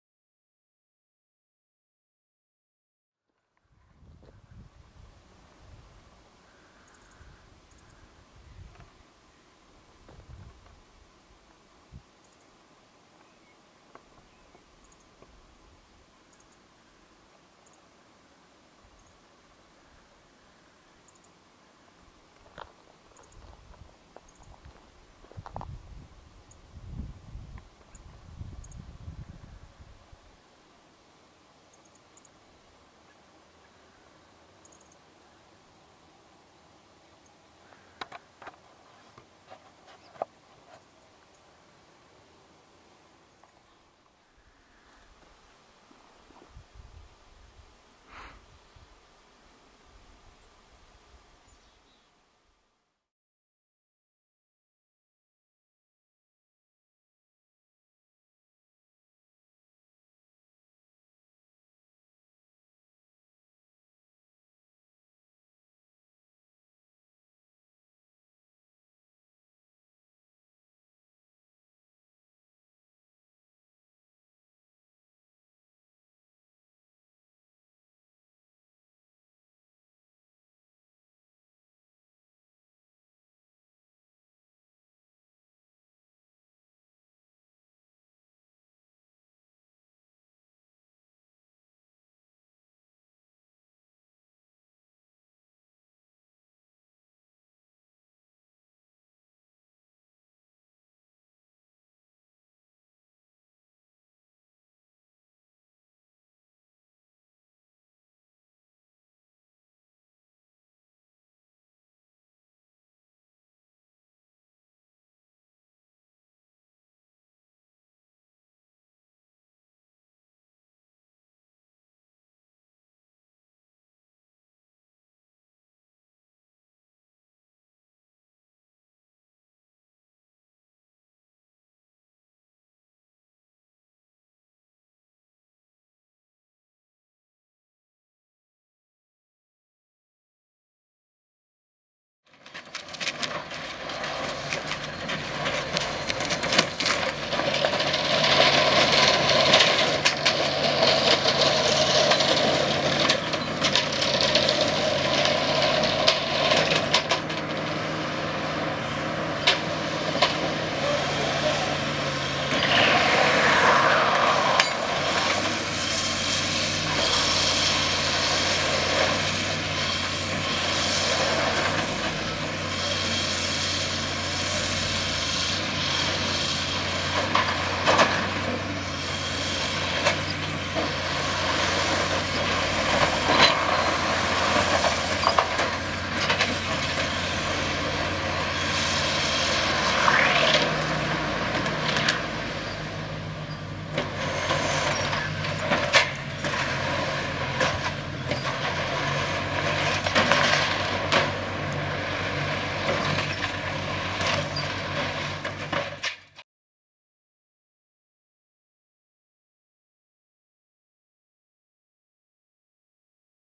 Canon SX20is Test
I just got this and decided to test it out and see if it is really better than my canon zr850 DV.